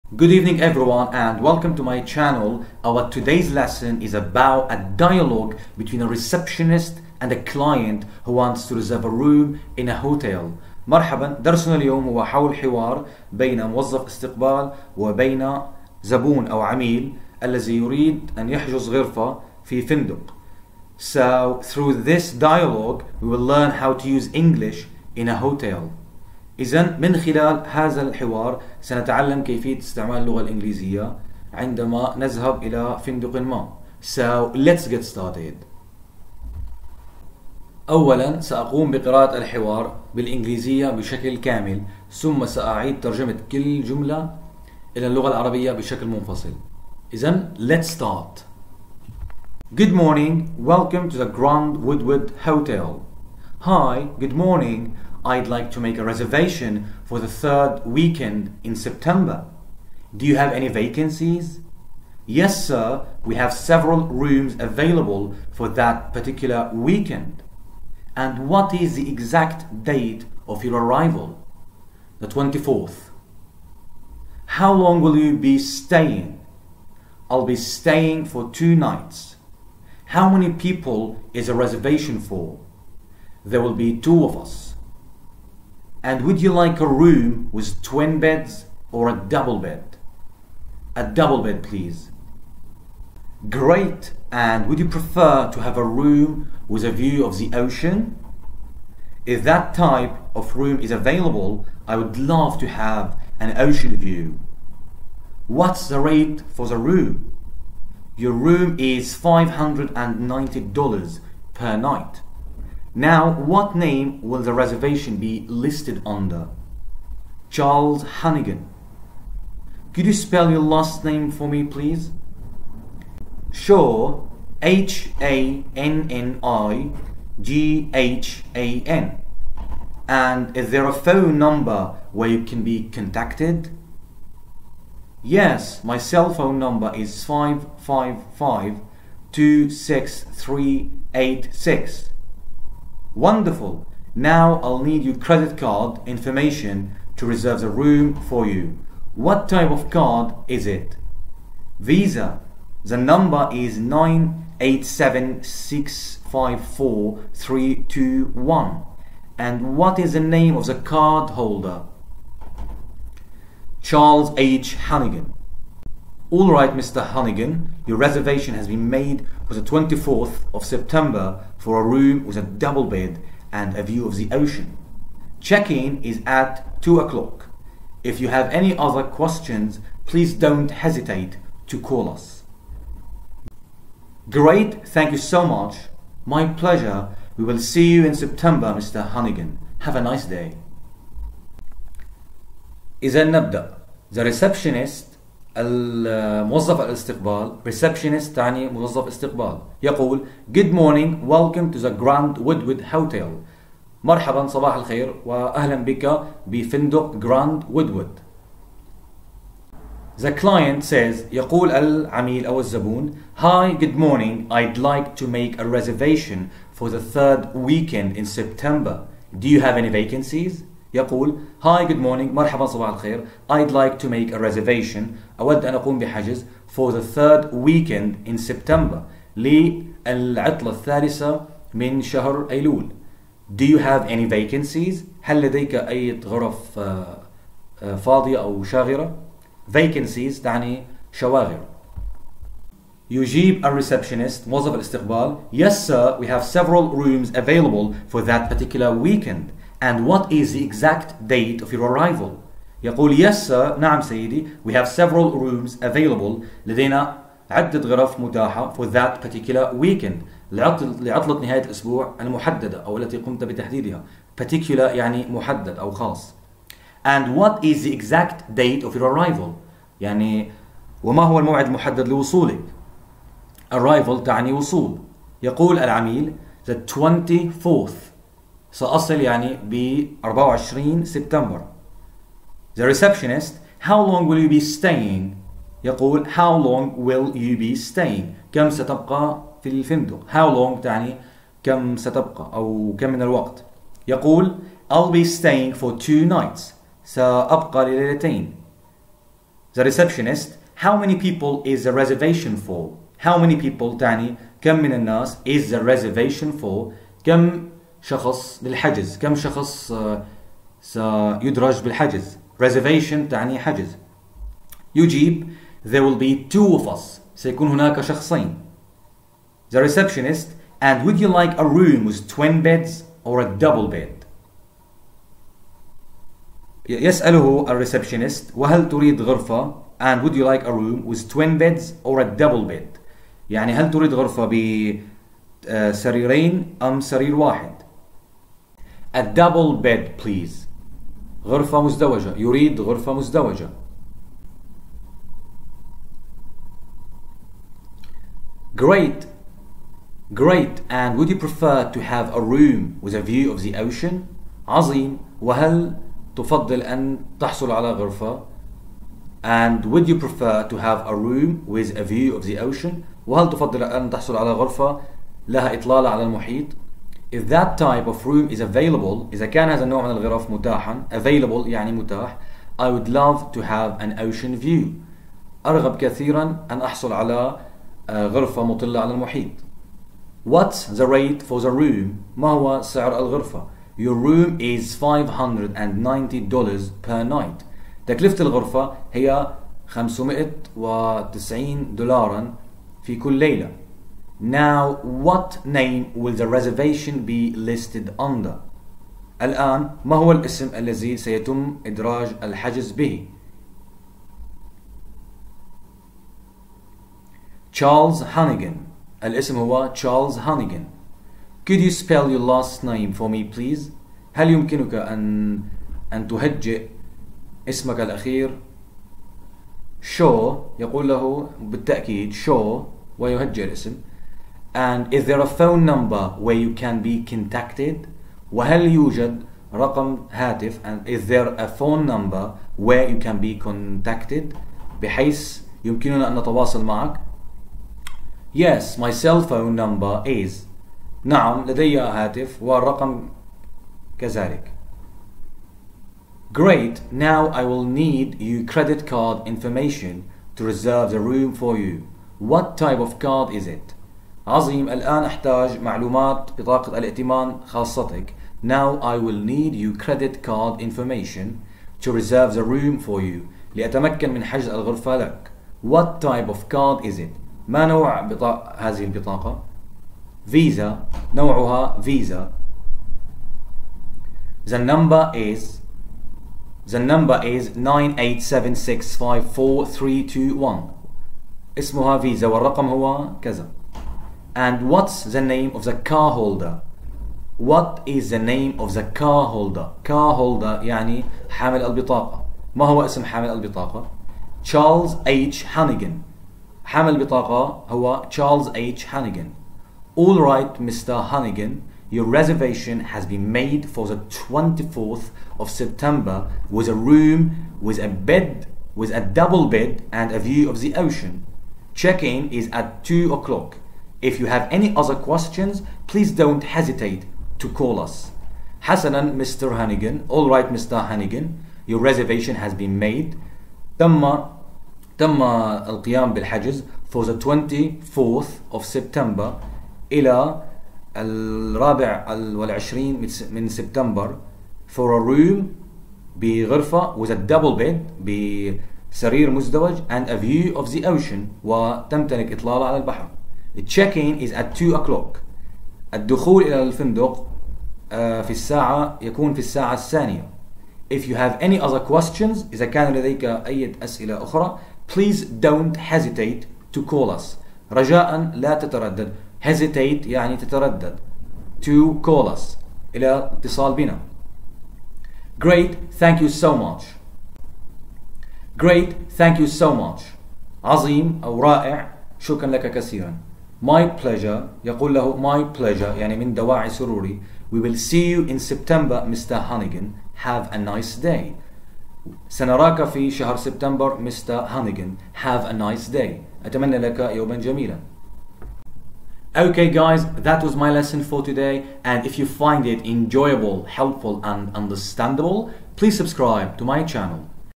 تعلم كيفية التحدث في الفندق بالإنجليزية بشكل احترافي!!
شاهد حوار بين موظف الاستقبال وبين شخص يود القيام بحجز في الفندق.
هذا الفيديو سوف يساعدك على إتقان كيفية التحدث والتعامل في الفندق باللغة الانجليزية.
شكراً جزيلاً لكم